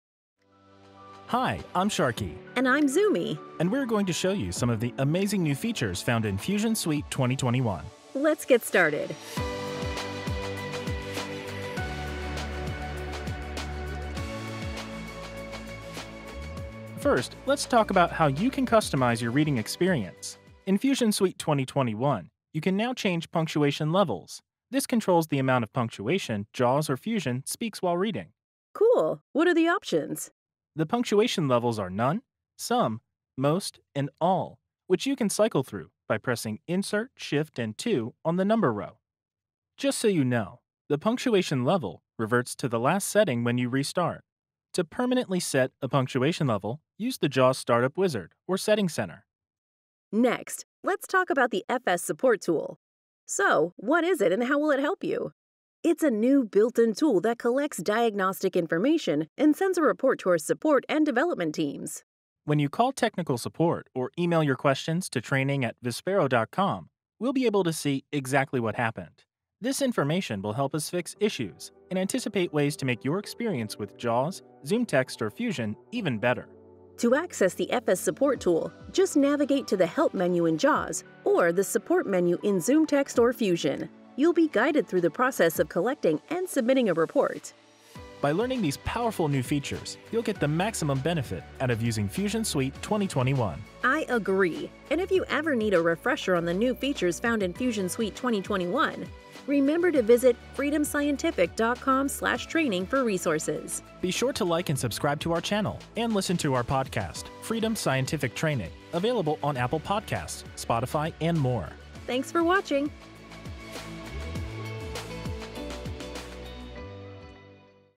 More New Features: Fusion Suite 2021
Check out this video to learn about two new features in Fusion Suite 2021! We'll give you a brief overview of improvements including:
– Punctuation Level Control
– The brand new FS Support Tool